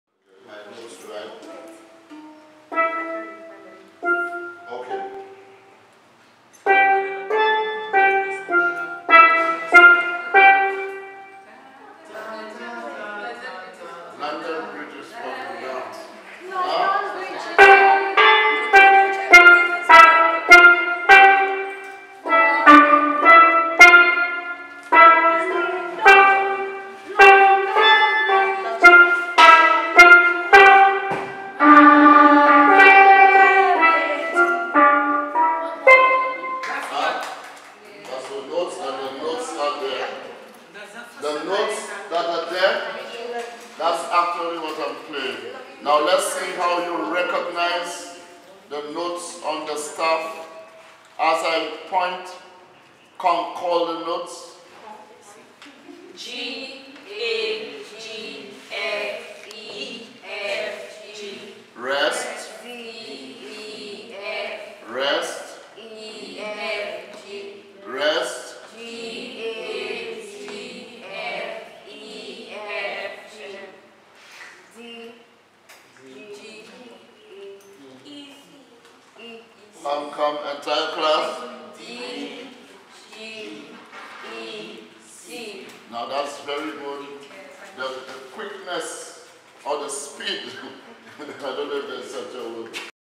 Steel Pan Music Theory Training
Gregory Piper tutors participants in the 2017 Steel Pan Training in Soufriere.  The students are being instructed in the rudiments of Music Theory, with an emphasis on the Steel Pan.  The training exercise is organised by the Cultural Development Foundation and is open to interested persons in and around Soufriere.  Participants  from neighbouring Canaries as well as Soufriere are hoping that the four (4) week Summer Programme will assist them in developing their skills in reading music and playing the steel pan.